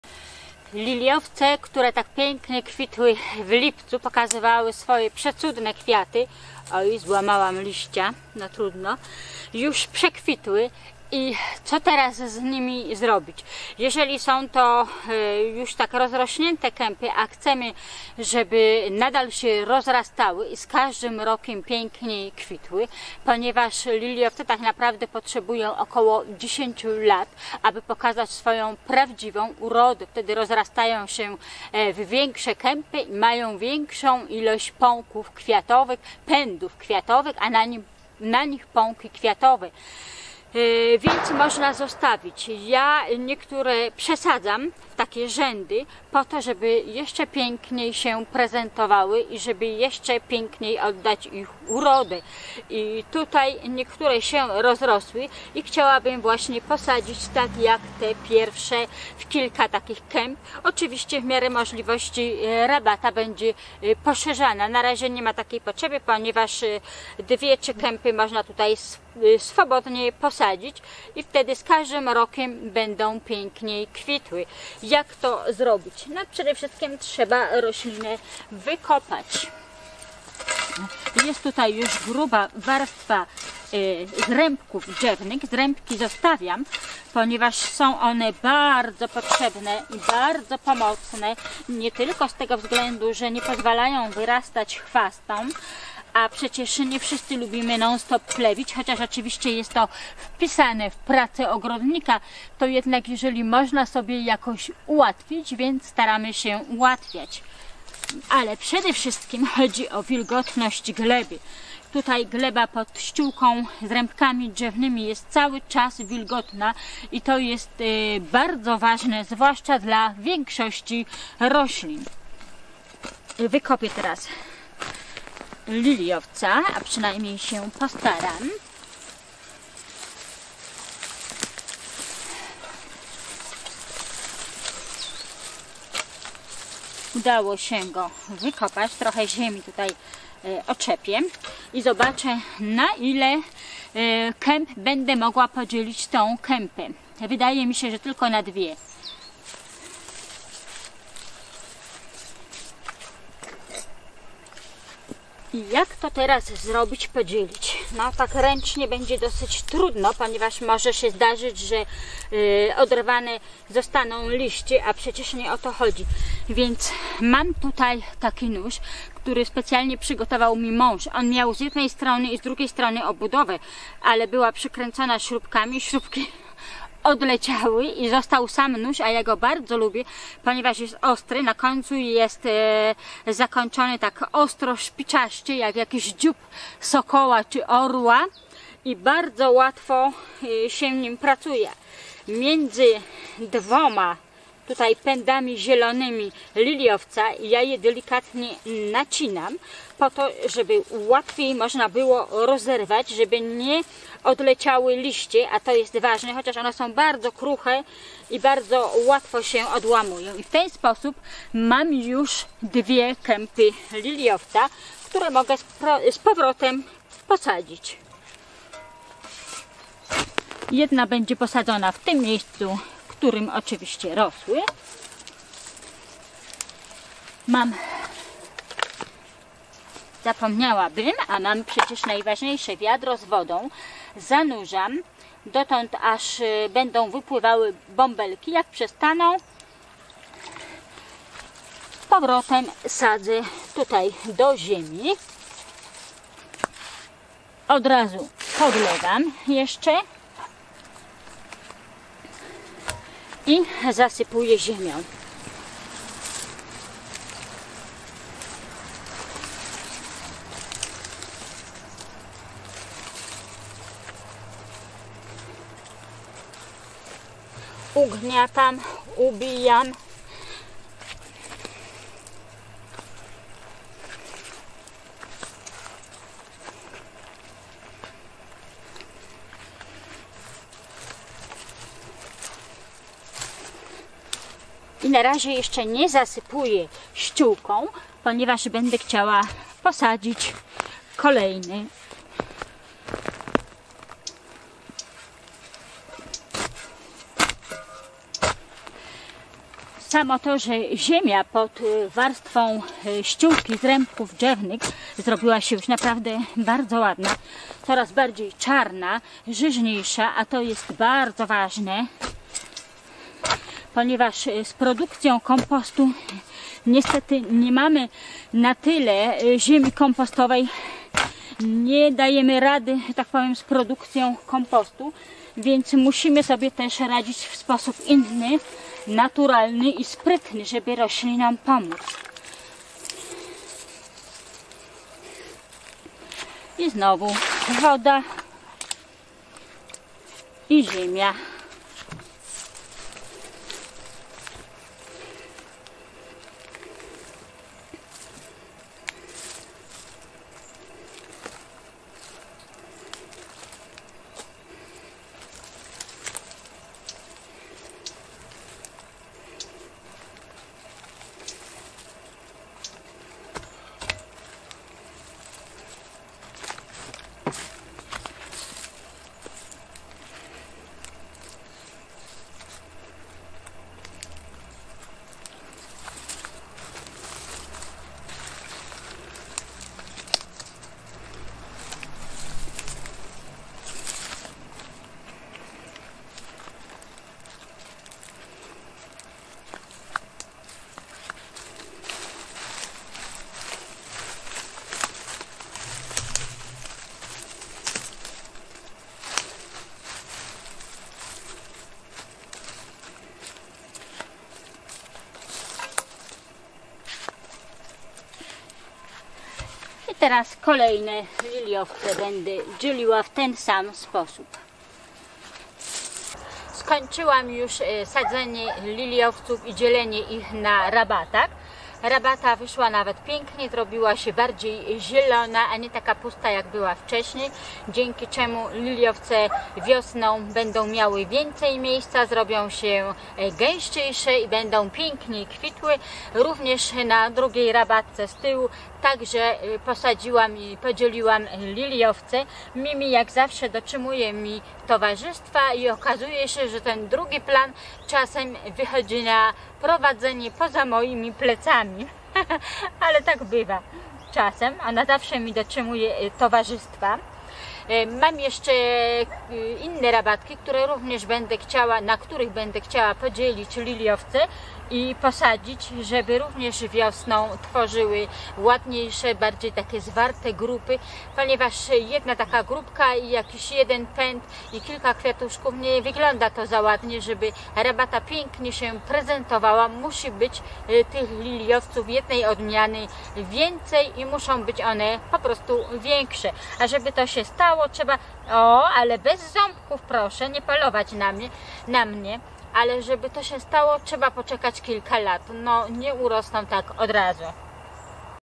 Liliowce przekwitły i co teraz?
Liliowce przekwitły i co teraz? Liliowce po posadzeniu powinno się przyciąć, ale nie jest to konieczne. Uprawę i nawożenie liliowców przedstawiałam we wcześniejszych filmach.
Bardzo serdecznie dziękuję za subskrypcje i liczne polubienia mojego kanału:) To cudownie mieć tak wspaniałych widzów, dziękuję.